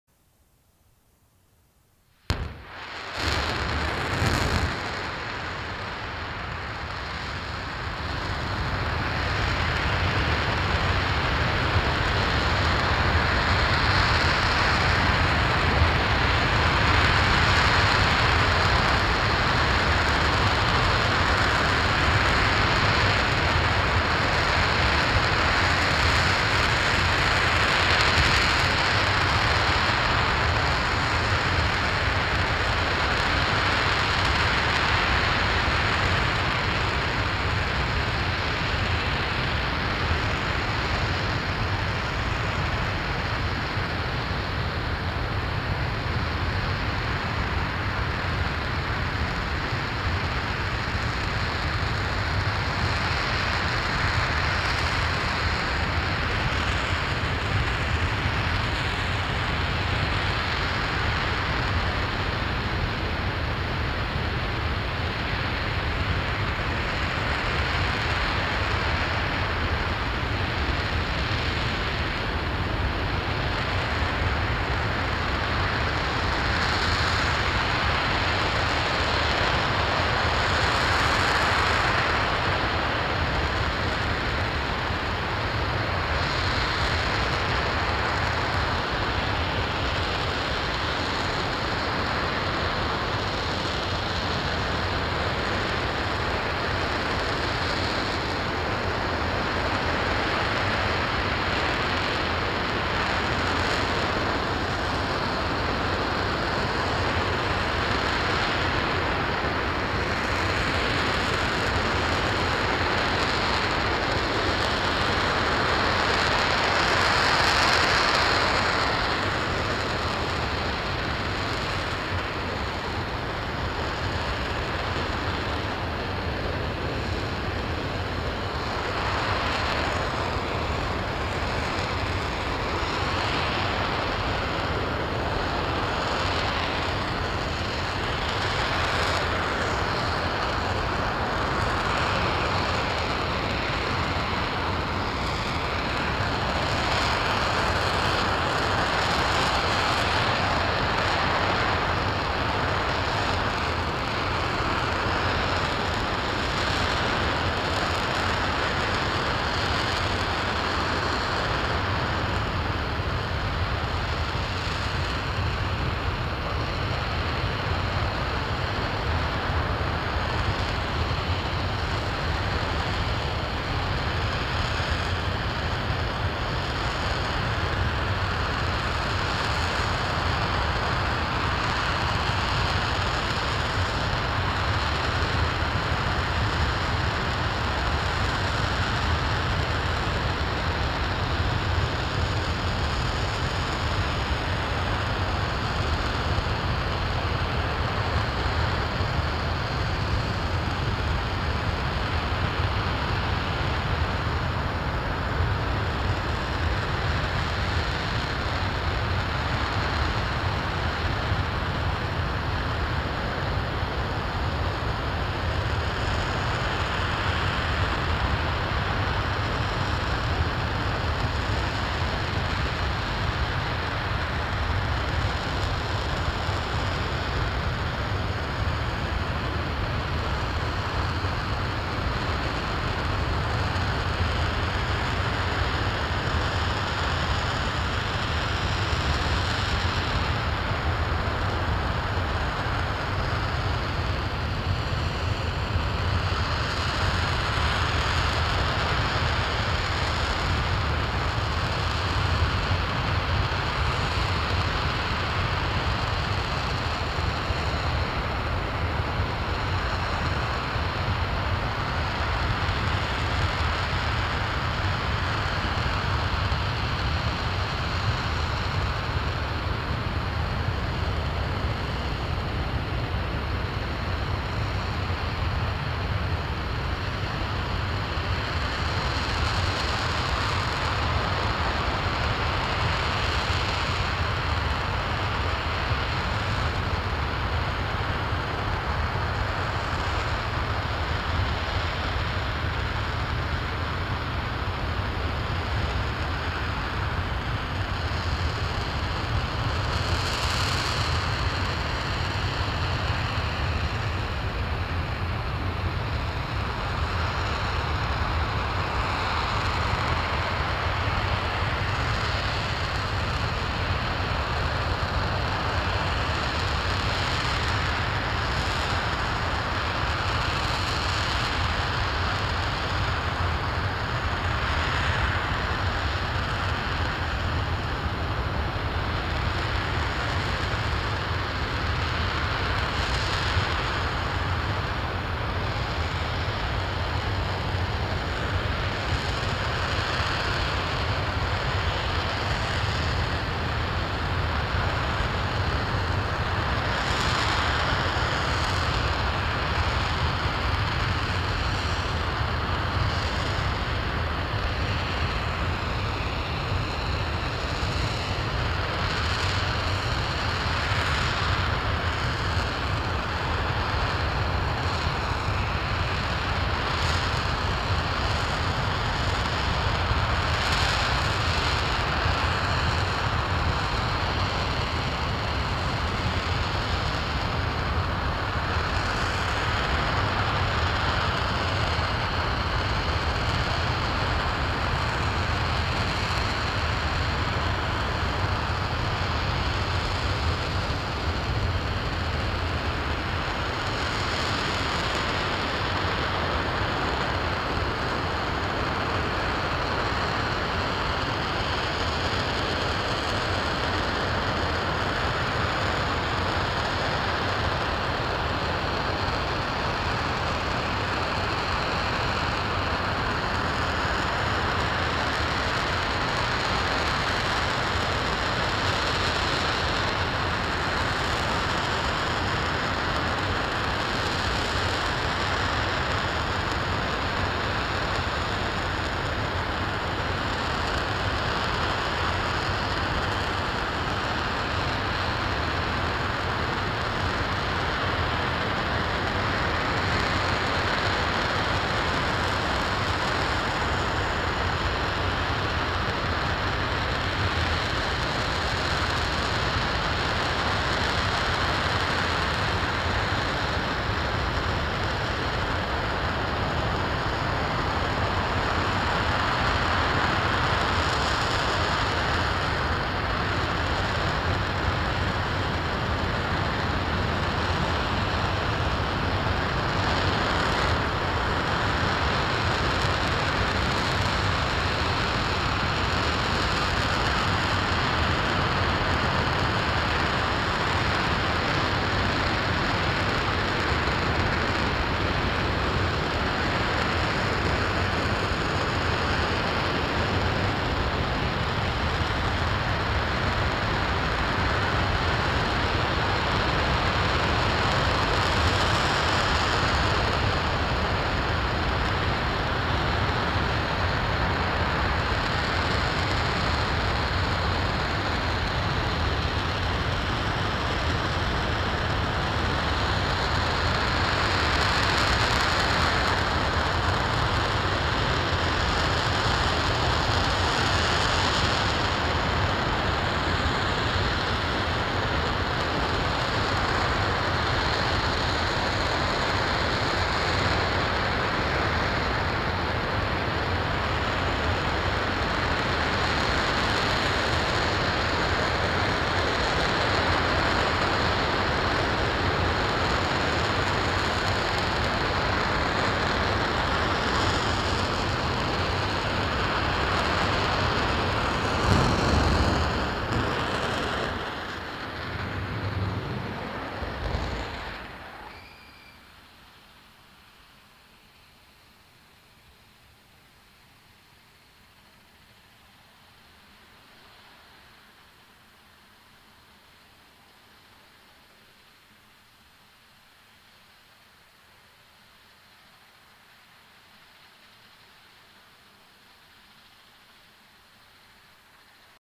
Raw Power! NASA fires up Artemis moon rocket engine for 550 second test
The Space Launch System rocket's RS-25 engine was fired up on Oct. 17, 2023 for the “final round of certification testing ahead of production of an updated set of the engines," according to NASA. The test occurred at the space agency’s Stennis Space Center.

Credit: NASA Stennis Space Center
🚀Help keep this channel going!🚀
🚀 Happy Exploring!!!🚀
NEW NASA Halloween Costumes!
SpaceX merch & Gift Ideas!